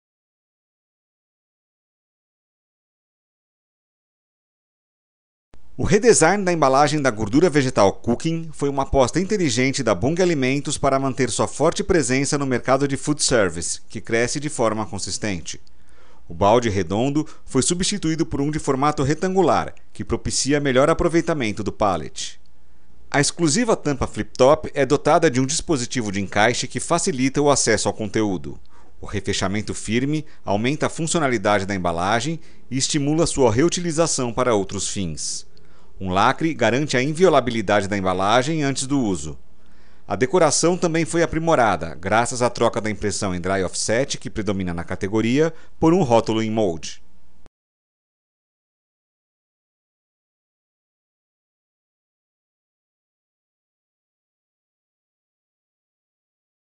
Gordura Vegetal Cukin
Case vencedor do Prêmio Grandes Cases de Embalagem 2015

Case
Gordura Vegetal Cukin 14,5kg

Usuário
Bunge Alimentos

Fornecedores Citados
Klabin
Mais Packing
Real Plastic

Veja mais sobre este case e conheça os outros vencedores desta edição do Prêmio